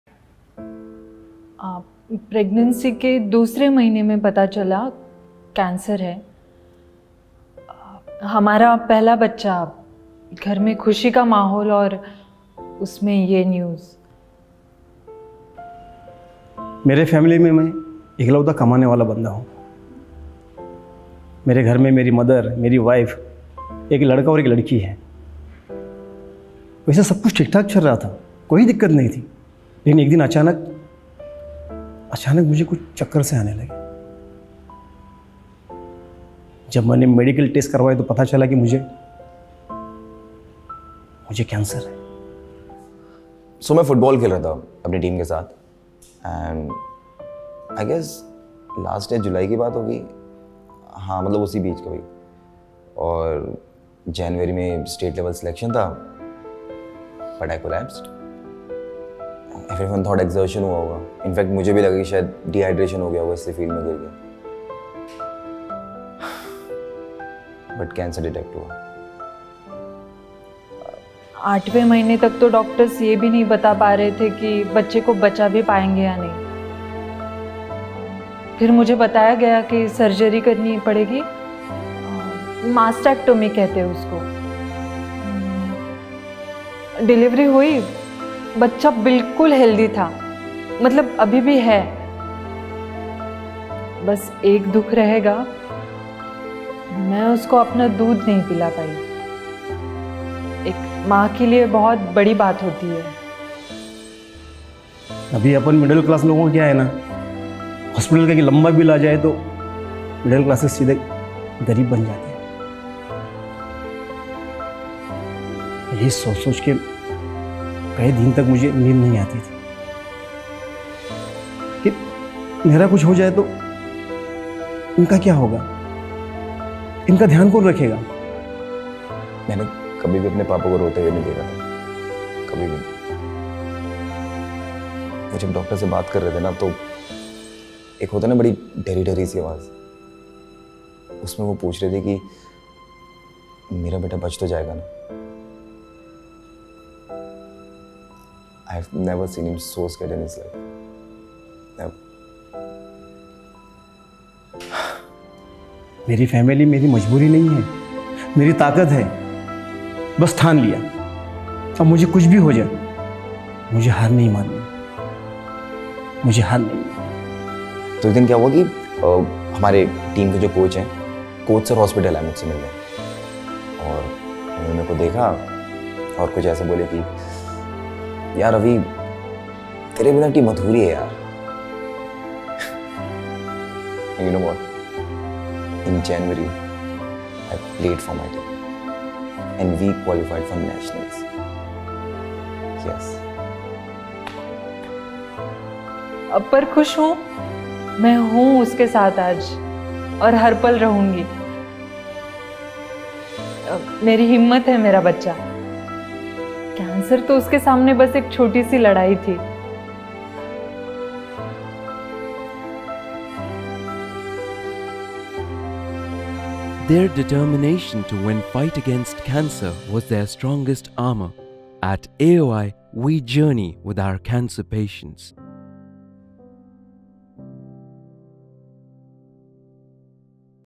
National Cancer Survivors Day | American Oncology Institute
An invincible determination can accomplish almost anything. On this National Cancer Survivors Day, we want to send a message to everyone who is battling against Cancer, that your determination to fight against cancer is your strongest armour. Never give up! Lets celebrate life of Cancersurvivors.